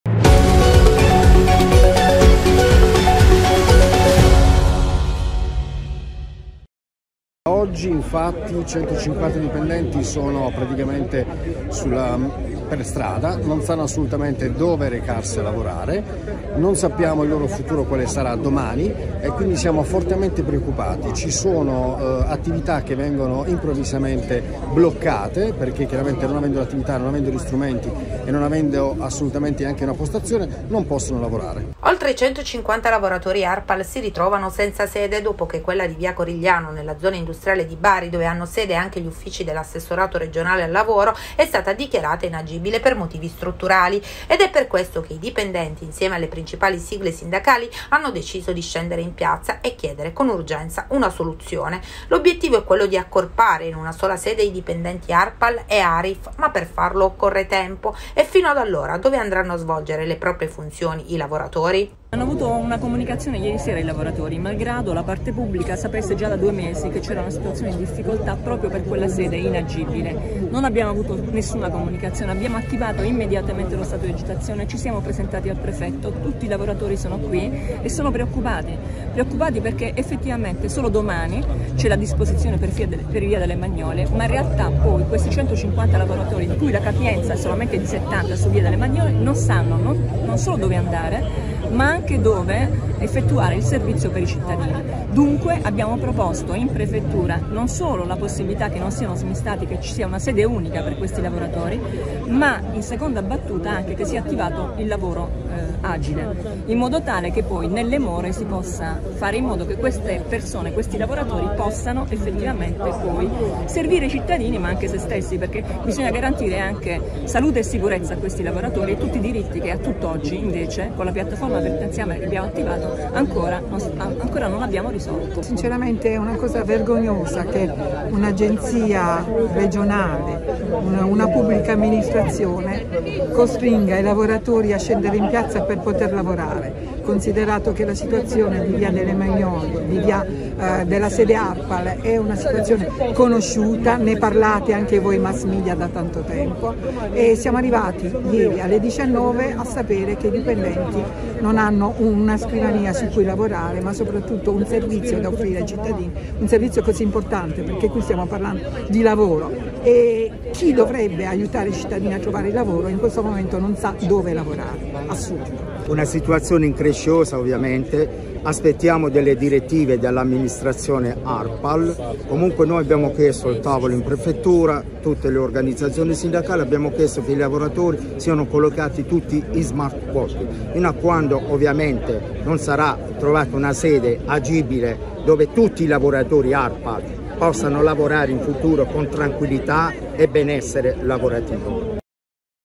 BARI | La sede Arpal dichiarata inagibile. 150 lavoratori senza una "casa" scendono in piazza a Bari
Dipendenti affiancati dalle sigle sindacali